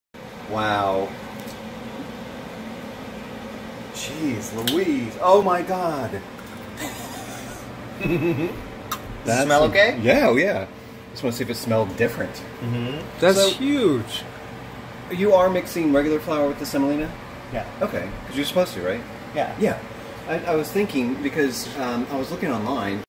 Curt breaking open a goose egg
Here we are starting to make our pasta using goose eggs. We were really surprised how BIG the yoke and egg whites were.